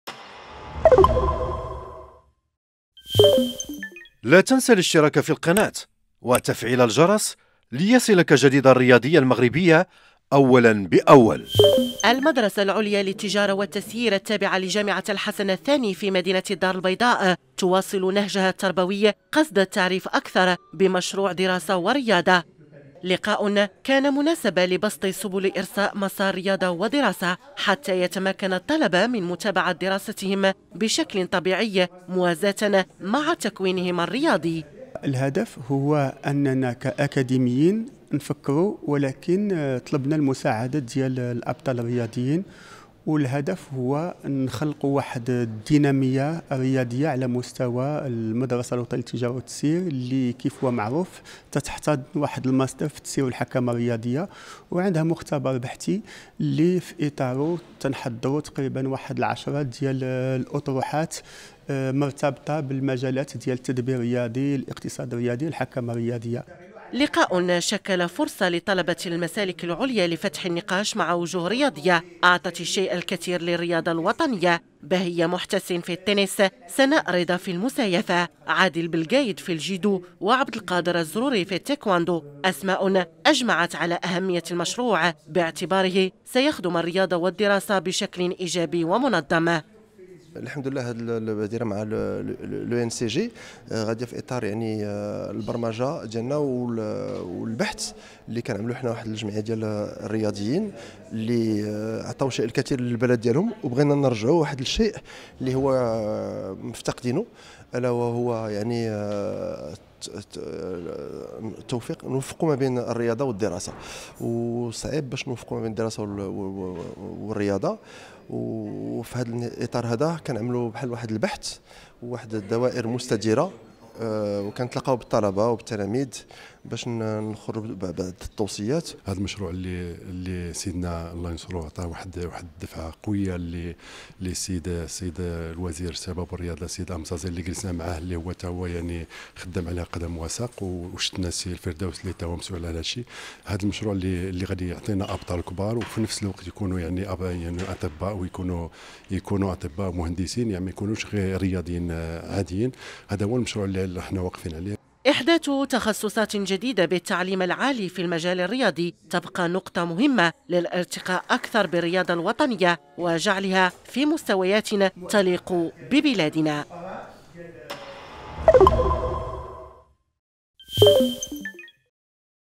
روبورتاج الرياضية|الدار البيضاء|المدرسة العليا للتجارة و التسيير التابعة لجامعة الحسن الثاني
روبورتاج الرياضية|الدار البيضاء|المدرسة العليا للتجارة و التسيير التابعة لجامعة الحسن الثاني تواصل نهجها التربوي قصد التعريف أكثر بمشروع دراسة و رياضة